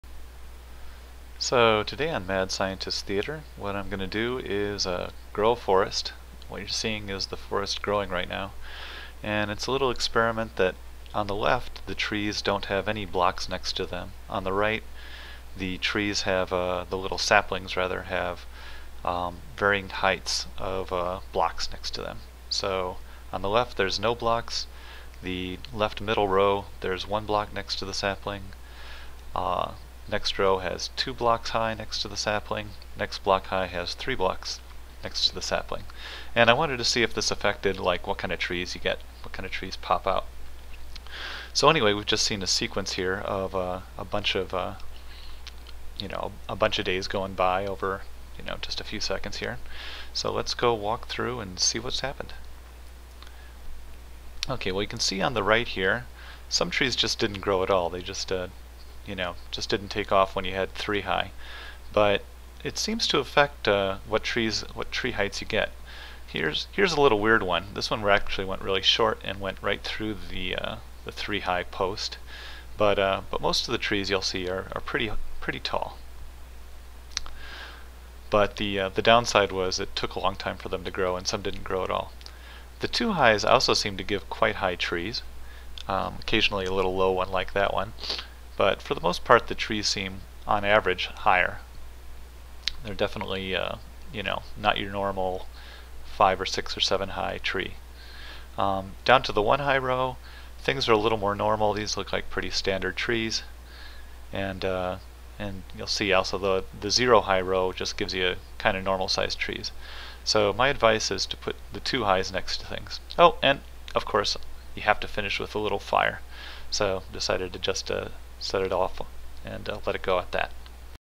Tree Growth Experiment
Four rows of saplings: on the left each is planted on flat ground, the next row each is planted next to a single block, next to the right two blocks, and the rightmost each is planted next to a column three blocks high.

I also tried (not shown here) putting just one block, two, and three on the ground in a circle around each sapling (i.e., not stacked up, just one block north, east, and south) - this didn't have any effect.

Note: from what I can tell, if you put a column of dirt next to a sapling and apply bone meal to the sapling, the sapling won't always instantly grow (on flat ground, it always does grow instantly). I think the code "rolls dice" and if the tree is deemed tall enough, it will then grow, else the bone meal is wasted.

If you want to grow trees in a tree farm, here's a less-hideous way to do so